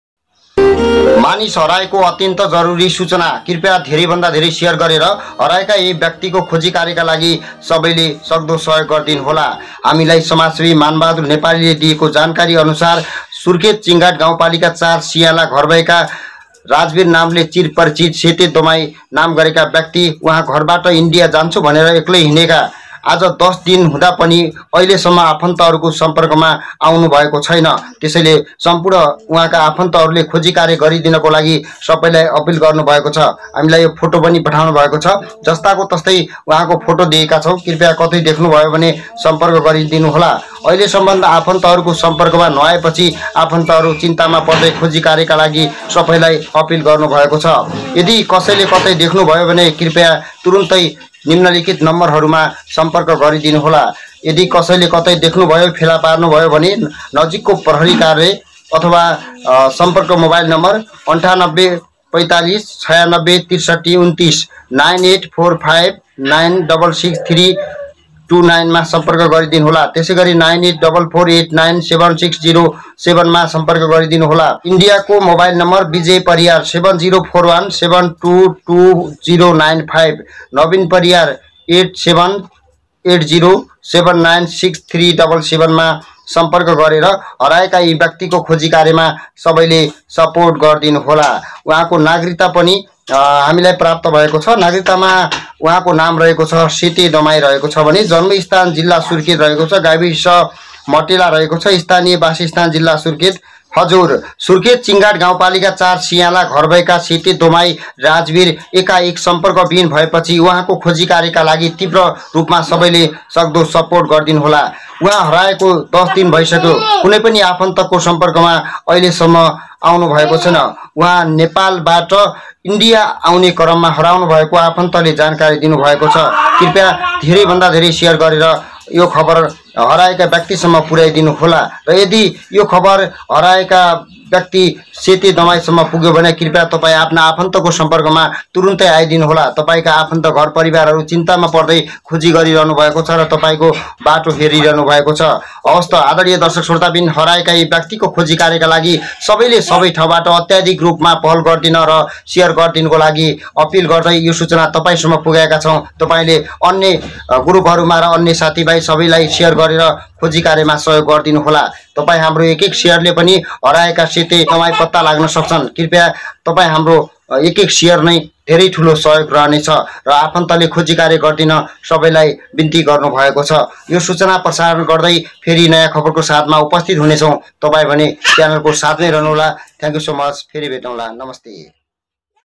Missing Riport Surkhet सुर्खेत का सेते दमाई भारत जान्छु भन्दै बेपत्ता, खोजिका लागी सेअर गर्नुहोला ।
Nepal News77
NayaKhabarAsyo
ManishHarayako Duchana Update